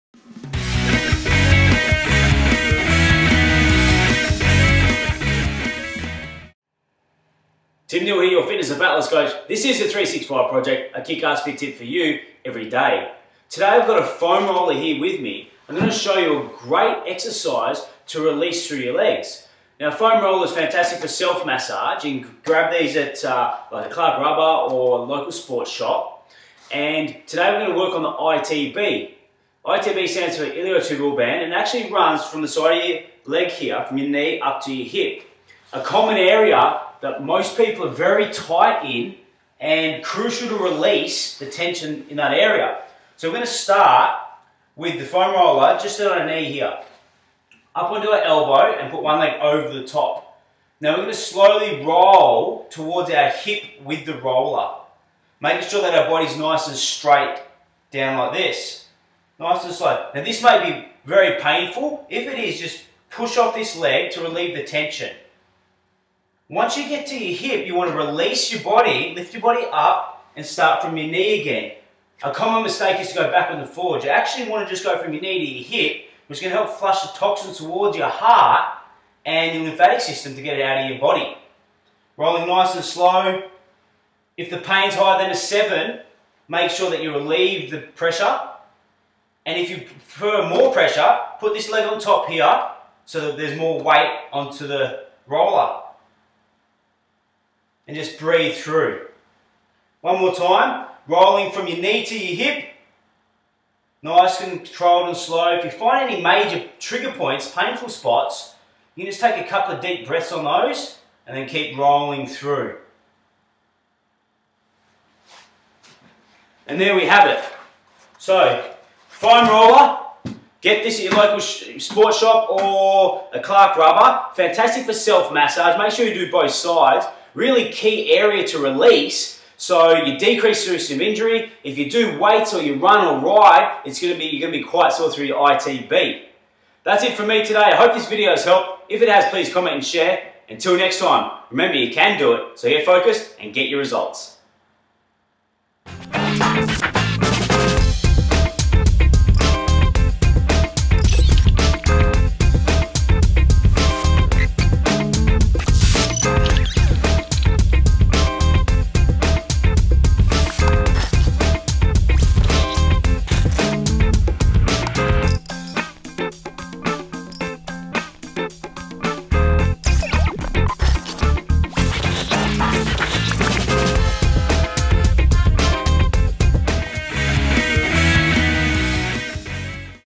365 Project- Looser Legs
Looser Legs with ITB Rolling - Tim Neal fitness expert shows you how to get Looser Legs using a foam roller. Highly effective, start doing this today.